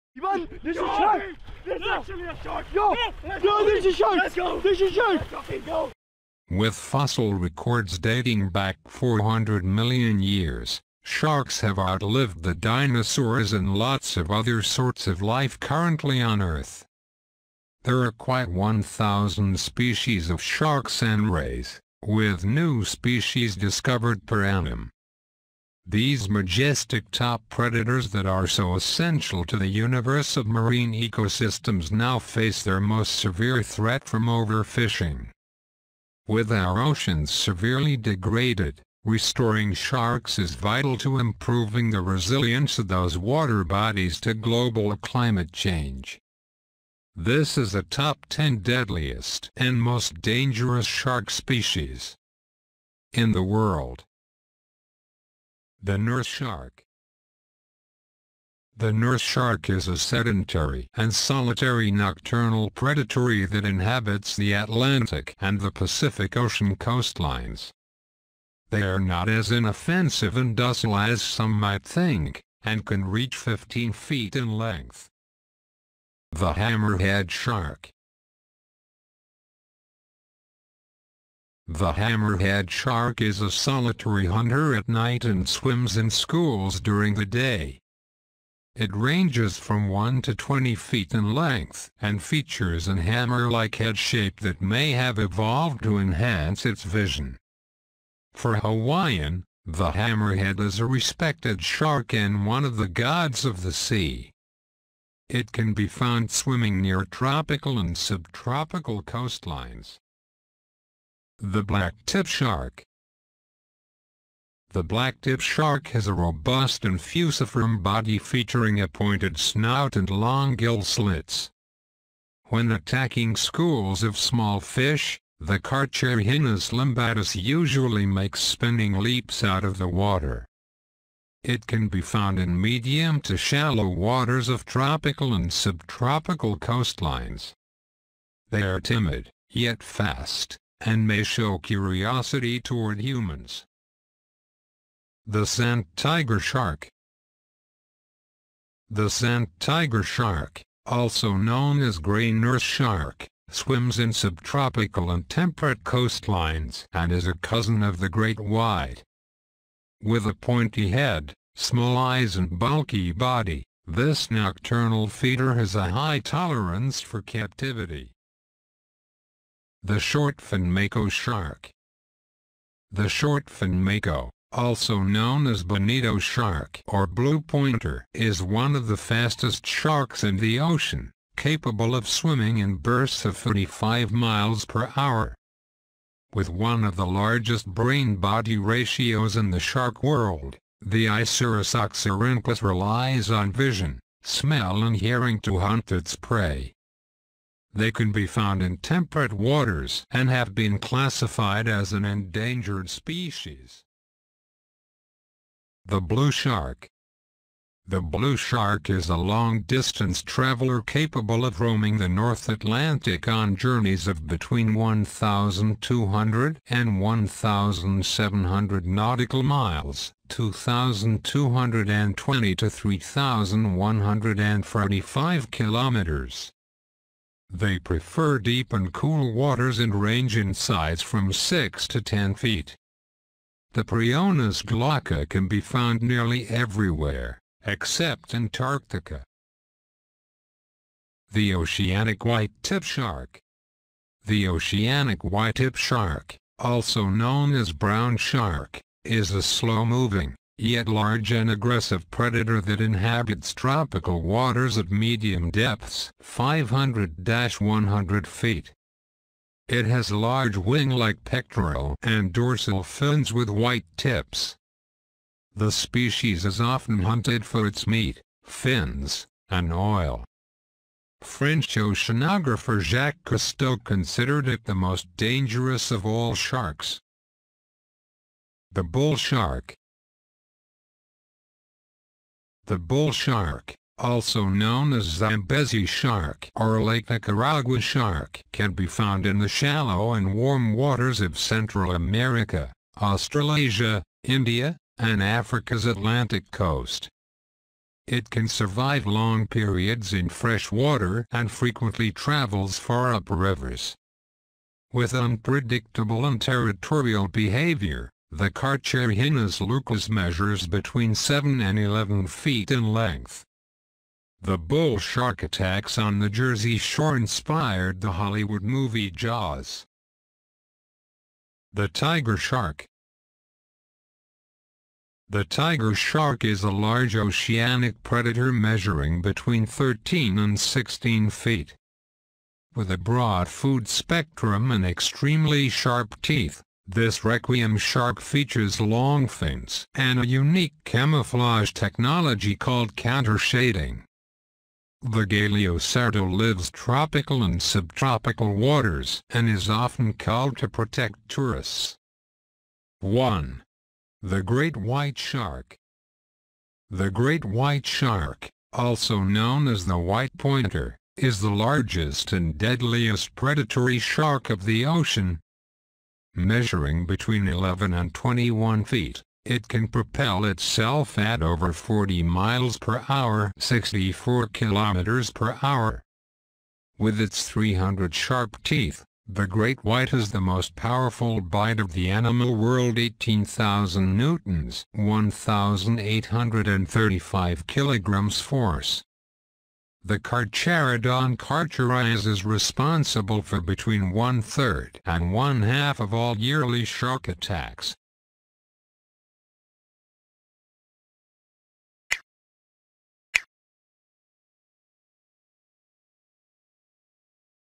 The deadliest and most dangerous shark species in the world
With fossil records dating back 400 million years, sharks have outlived the dinosaurs and many other forms of life currently on earth. There are more than 1,000 species of sharks and rays, with new species discovered every year.
These majestic top predators that are so essential to the natural order of marine ecosystems now face their most severe threat from overfishing. Many species are threatened with extinction, with some families of rays such as sawfishes in peril. While sharks and rays have been an irreplaceable resource for coastal communities in the developing world for centuries, this unique balance is in danger of being lost forever.
The deadliest and most dangerous shark species

Of all 375 shark species, only about 12 are considered potentially dangerous to humans.

1. The Great White Shark

The great white shark, also known as the "white pointer," is the largest and deadliest predatory shark of the ocean.
Measuring between 11 and 21 feet, it can propel itself at over 40 miles per hour (64 kilometers per hour).
With its 300 sharp teeth, the great white has the most powerful bite of the animal world - 18,000 Newtons (1,835 kilograms force).
The Carcharodon carcharias is responsible for between one-third and one-half of all yearly shark attacks;

2. The Tiger Shark

The tiger shark is a large oceanic predator measuring between 13 and 16 feet.
With a broad food spectrum and extremely sharp teeth, this requiem shark features long fins and a unique camouflage technology called countershading.
The Galeocerdo lives tropical and subtropical waters and is often culled to protect tourists.

3. The Bull Shark

The bull shark, also known as "Zambezi shark" or "Lake Nicaragua shark" can be found in the shallow and warm waters of Central America, Australasia, India, and Africa's Atlantic coast.
It can survive long periods in fresh water and frequently travels far up rivers. With unpredictable and territorial behavior, the Carcharhinus leucas measures between 7 and 11 feet in length.
The bull shark attacks on the Jersey Shore inspired the Hollywood movie "Jaws."

4. The Oceanic Whitetip Shark

The oceanic whitetip shark, also known as "brown shark," is a slow-moving, yet large and aggressive predator that inhabits tropical waters at medium depths (500-100 feet).
It has large wing-like pectoral and dorsal fins with white tips. The species is often hunted for its meat, fins, and oil.
French oceanographer Jacques Cousteau considered it "the most dangerous of all sharks."

5. The Blue Shark

The blue shark is a long-distance traveler capable of roaming the North Atlantic on journeys of between 1,200 and 1,700 nautical miles (2,220 to 3,145 kilometers).
They prefer deep and cool waters and range in size from 6 to 10 feet. The Prionace glauca can be found nearly everywhere, except Antarctica.

6. The Shortfin Mako Shark

The shortfin mako, also known as "bonito shark" or "blue pointer" is one of the fastest sharks in the ocean, capable of swimming in bursts of 45 miles per hour.
With one of the largest brain-body ratios in the shark world, the Isurus oxyrinchus relies on vision, smell and hearing to hunt its prey.
They can be found in temperate waters and have been classified as an endangered species.

7. The Sand Tiger Shark
The sand tiger shark, also known as "grey nurse shark," swimsin subtropical and temperate coastlines and is a cousin of the great white.
With a pointy head, small eyes and bulky body, this nocturnal feeder has a high tolerance for captivity.

8. The Blacktip Shark
The blacktip shark has a robust and fusiform body featuring a pointed snout and long gill slits.
When attacking schools of small fish, the Carcharhinus limbatus usually makes spinning leaps out of the water.
It can be found in medium-to-shallow waters of tropical and subtropical coastlines. They're timid, yet fast, and may show curiosity toward humans.

9. The Hammerhead Shark
The hammerhead shark is a solitary hunter at night and swims in schools during the day.
It ranges from 1 to 20 feet in length and features an hammer-like head shape that may have evolved to enhance its vision.
For Hawaiian, the hammerhead is a respected shark and one of the gods of the sea. It can be found swimming near tropical and subtropical coastlines.

10. The Nurse Shark
The nurse shark is a sedentary and solitary nocturnal predatory that inhabits the Atlantic and the Pacific Ocean coastlines.
They're not as inoffensive and docile as some might think, and can reach 15 feet in length.